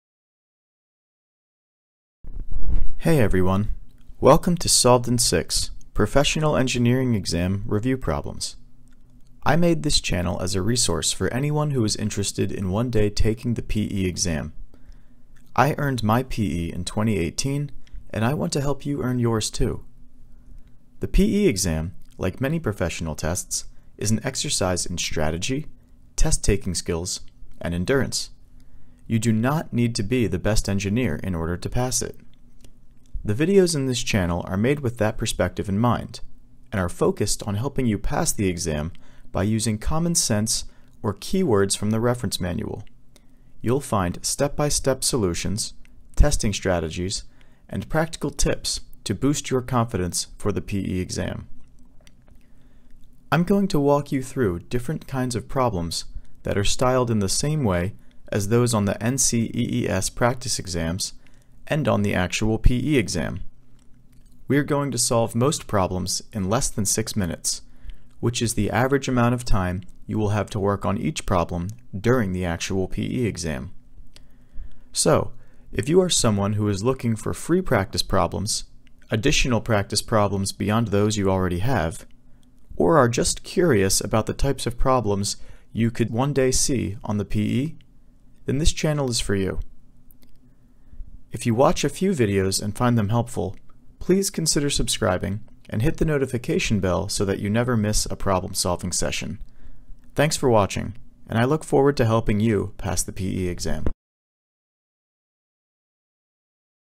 Welcome to Solved In 6!
Welcome to Solved In 6! This video is meant to give you a brief overview of what each Professional Engineering Practice Problem will be like.